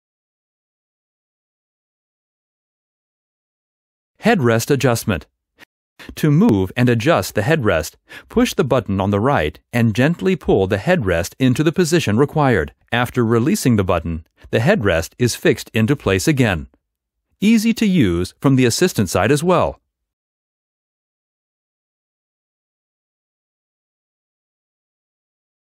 Instruction video: Primus 1058 Life - Adjust headrest
Everything you need to know about the operation and preparation of the Primus 1058 Life. More information can be found here: